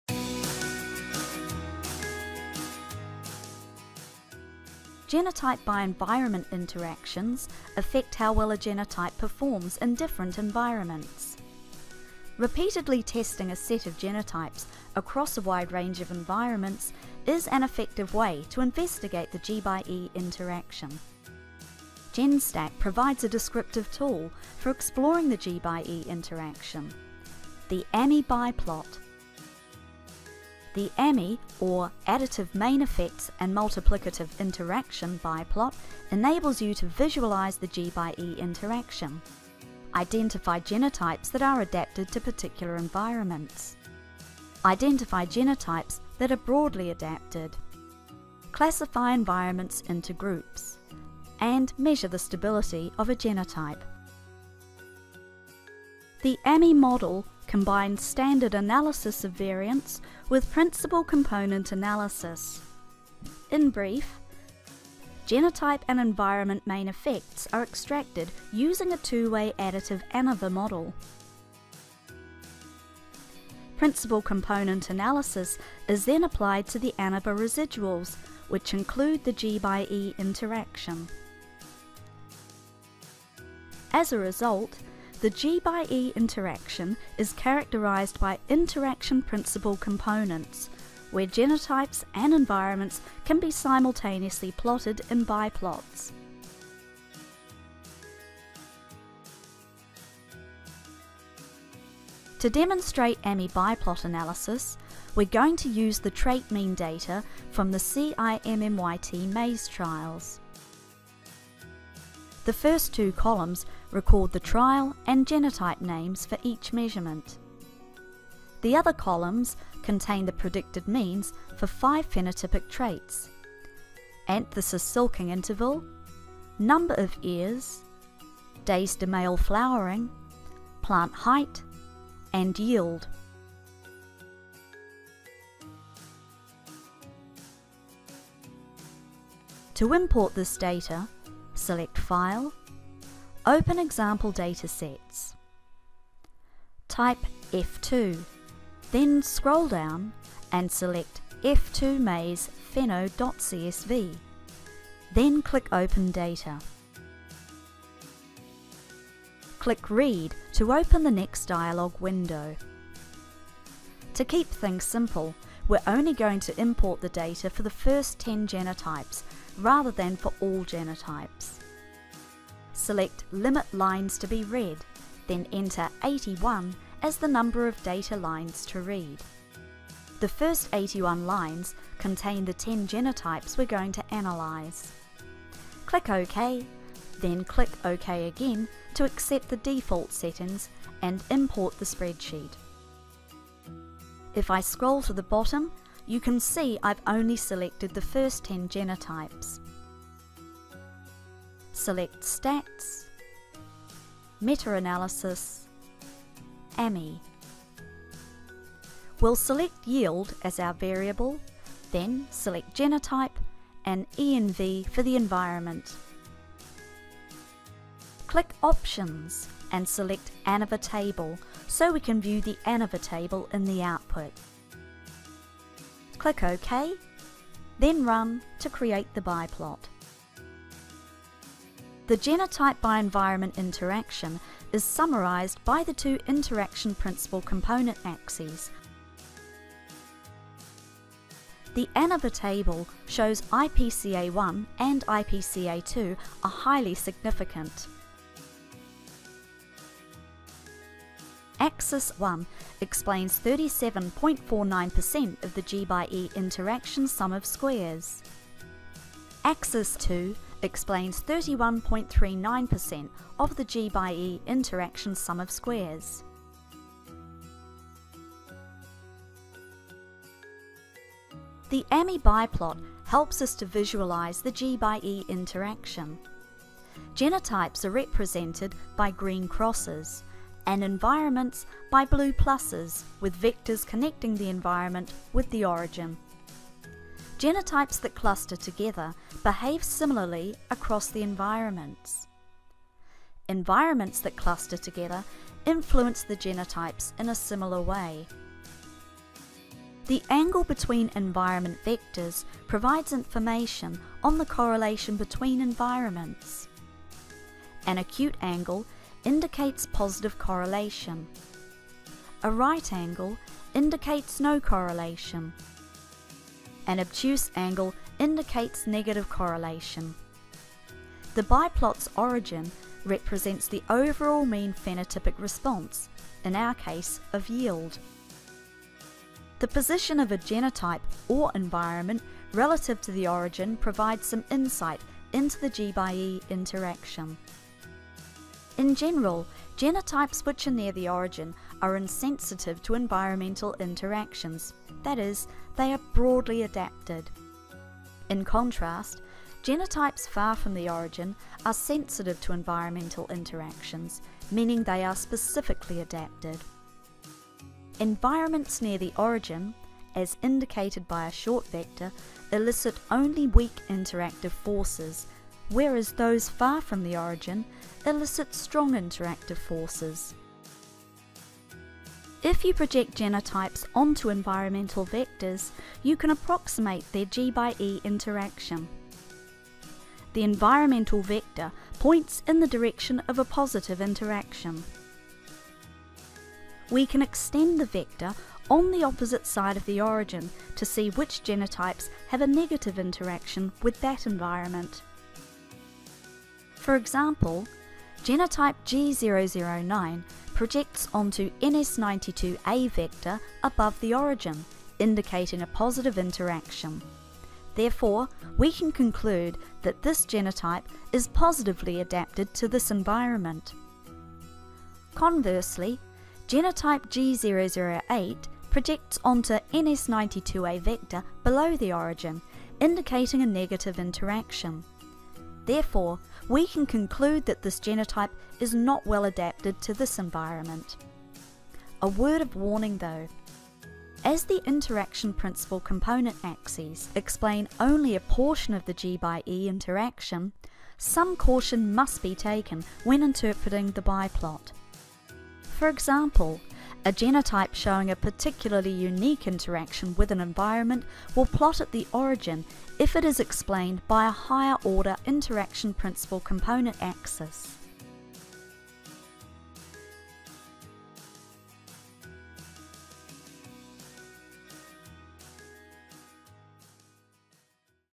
AMMI biplots in Genstat
Genstat provides a descriptive tool for exploring the GxE interaction; the AMMI biplot.